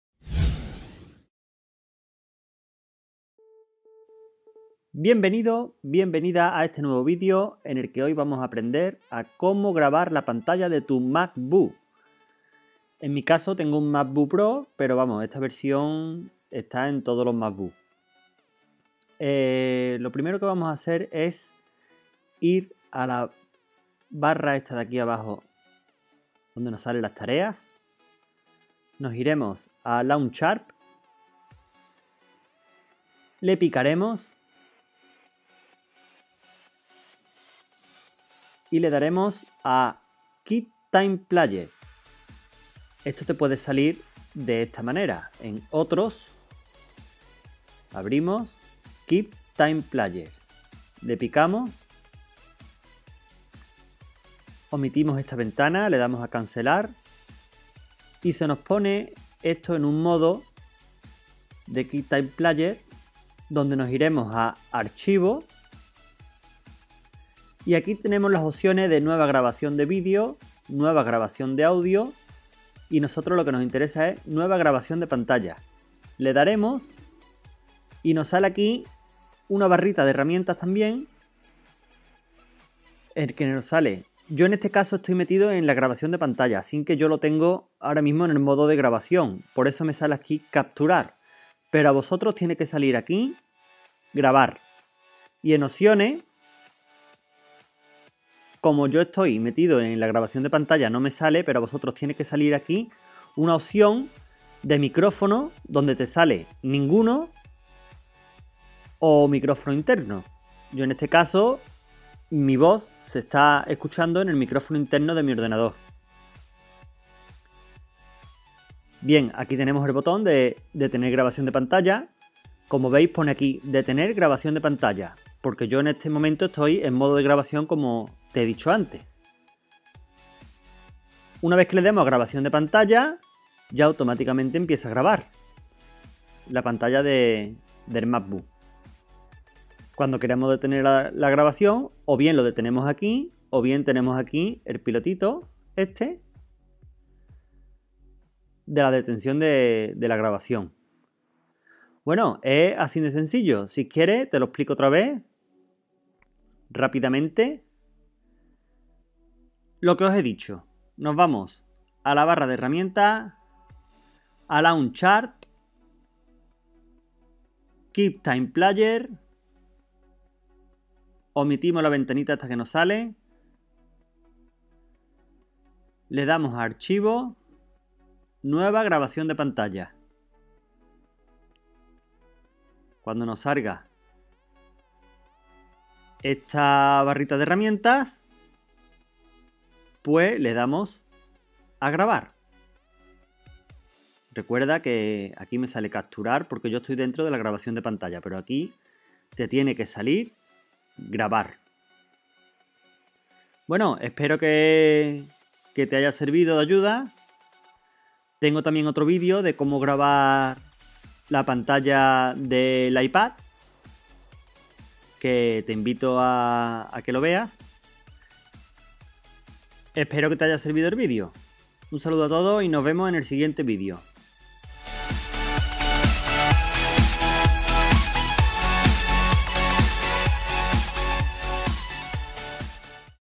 como grabar pantalla de macbook
En este video te muestro como grabar la pantalla de tu macbook, en otro video anterior ya muestro como grabar la pantalla de tu iPad, suscríbete a mi canal para no perderte nuevos videos.